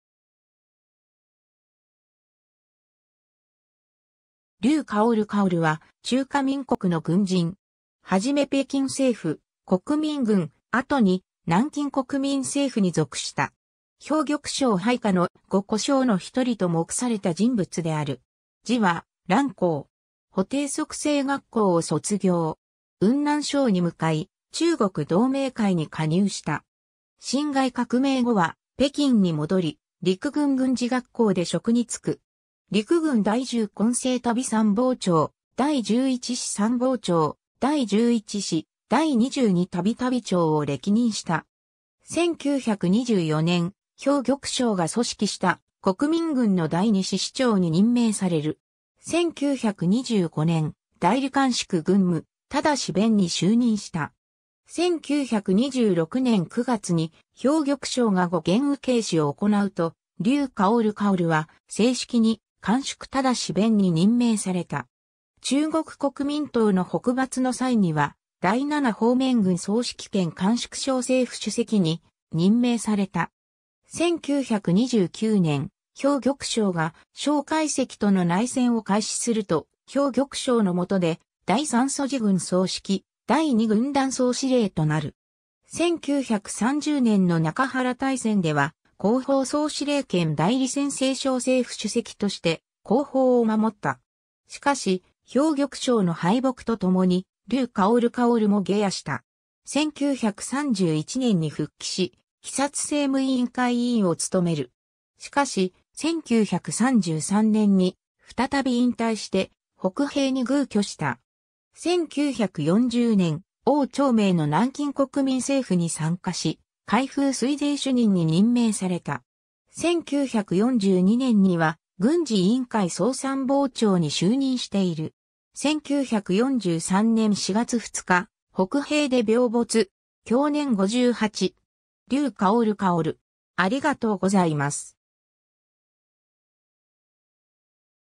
劉郁芬
劉 郁芬（りゅう いくふん）は中華民国の軍人。
初め北京政府、国民軍、後に南京国民政府（汪兆銘政権）に属した。
馮玉祥配下の「五虎将」の1人（他は張之江・鹿鍾麟・宋哲元・鄭金声）と目された人物である。
字は蘭江。
 保定速成学校を卒業。
雲南省に向かい、中国同盟会に加入した。
 辛亥革命後は北京に戻り、陸軍軍事学校で職に就く。
陸軍第10混成旅参謀長、第11師参謀長、第11師第22旅旅長を歴任した。
1924年（民国13年）、馮玉祥が組織した国民軍の第2師師長に任命される。
1925年（民国14年）、代理甘粛軍務督弁に就任した。
 1926年（民国15年）9月に馮玉祥が五原誓師を行うと、劉郁芬は正式に甘粛督弁に任命された。
中国国民党の北伐の際には、第7方面軍総指揮兼甘粛省政府主席に任命された。
1929年（民国18年）、馮玉祥が蔣介石との内戦を開始すると、馮玉祥の下で第3路軍総指揮、第2軍団総司令となる。
1930年（民国19年）の中原大戦では、後方総司令兼代理陝西省政府主席として後方を守った。
しかし、馮玉祥の敗北とともに、劉郁芬も下野した。
 1931年（民国20年）に復帰し、冀察政務委員会委員をつとめる。
しかし、1933年（民国22年）に再び引退して北平に寓居した。
 1940年（民国29年）、汪兆銘（汪精衛）の南京国民政府に参加し、開封綏靖主任に任命された。
1942年（民国31年）には、軍事委員会総参謀長に就任している。
 1943年（民国32年）4月2日、北平で病没。
享年58。
 劉郁芬 (1940年前後)